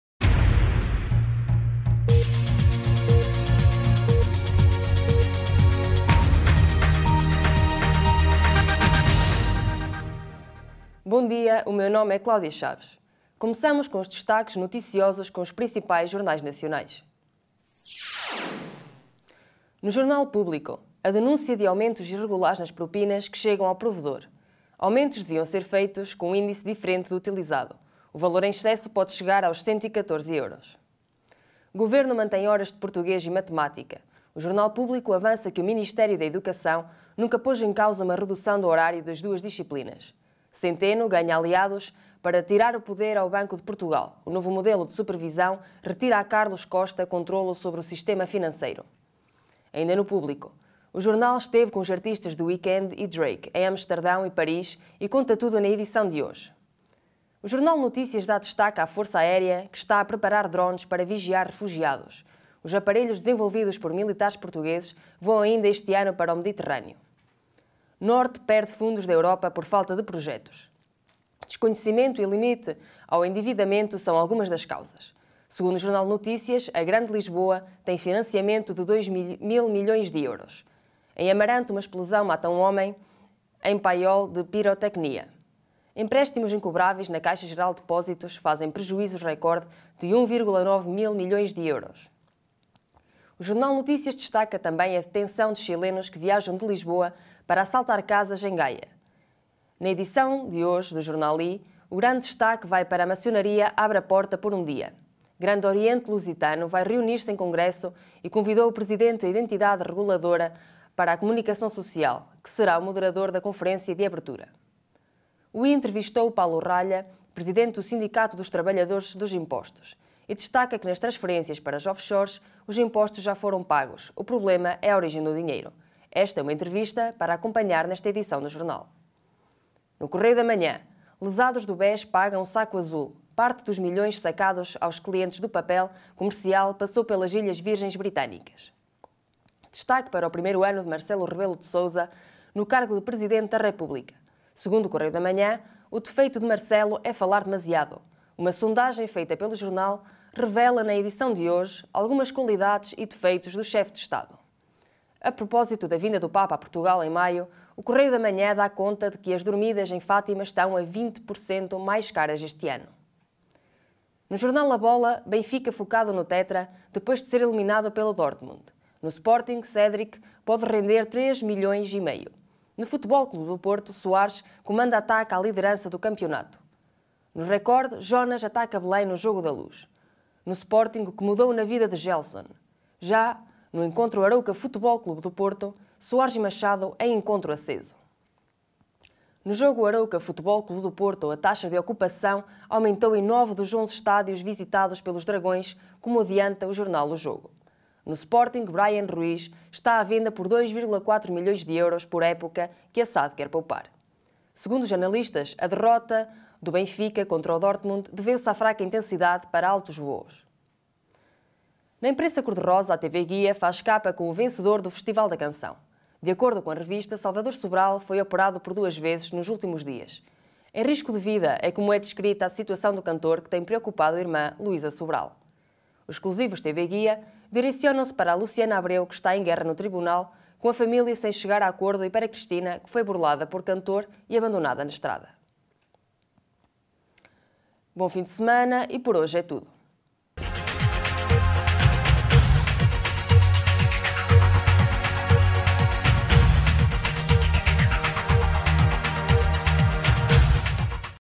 Diário 10/03/2017
As capas dos principais jornais e revistas do dia com Cláudia Chaves.